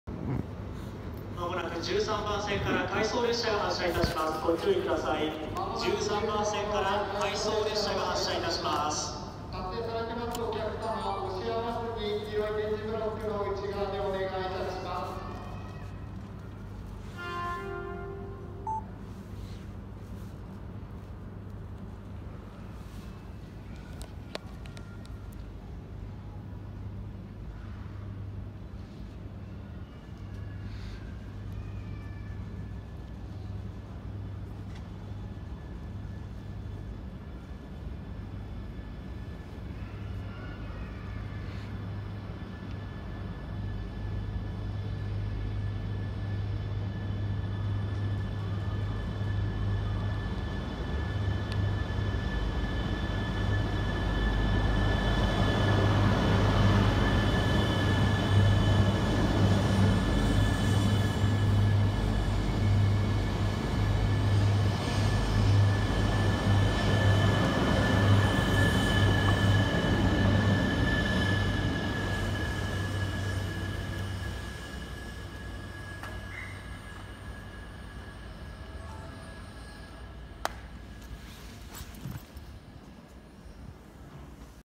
現美新幹線最後の新潟発車
すみません最初の方カメラを下に向けてたので真っ暗になってます

この動画はご自由にご利用いただけます